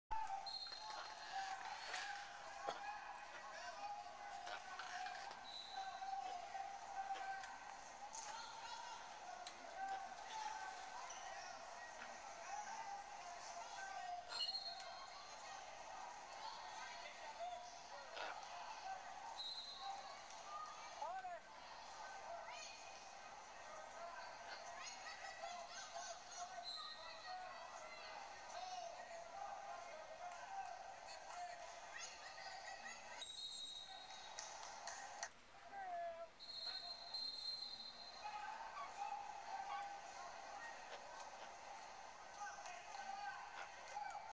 Sarah Phillips EUHS vs Andrea Avila STAGG 12-5-2009
Her 1st time wrestling she gets a 2nd place this is her 4th match at McNair Lady Eagles Tournament, 32 second pin